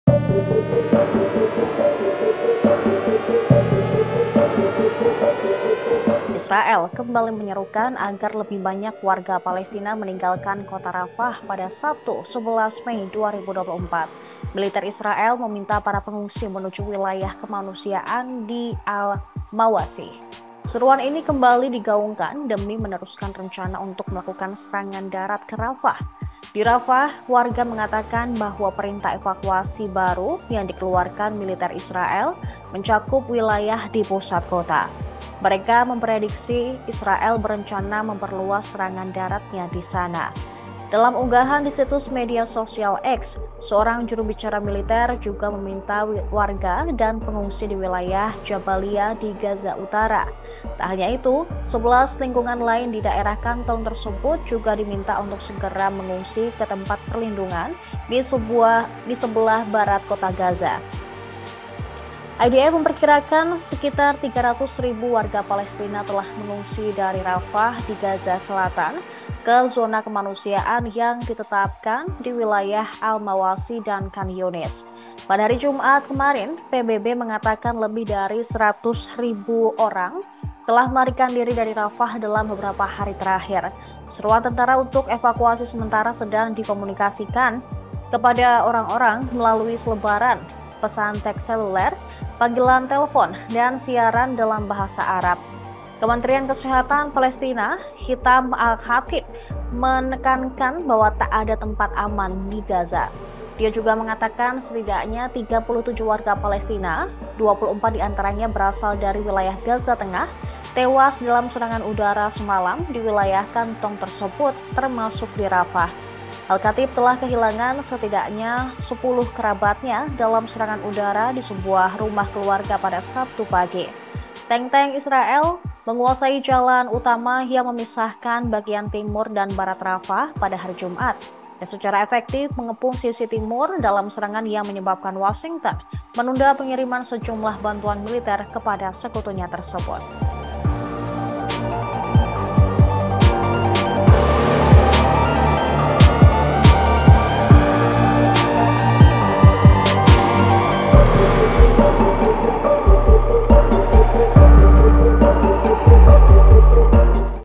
300.000 Orang Ngungsi, Israel Usir Paksa Warga Palestina di Rafah
BANGKAPOS.COM -  300.000 Orang Ngungsi, Israel Usir Paksa Warga Palestina di Rafah

VP: ASIF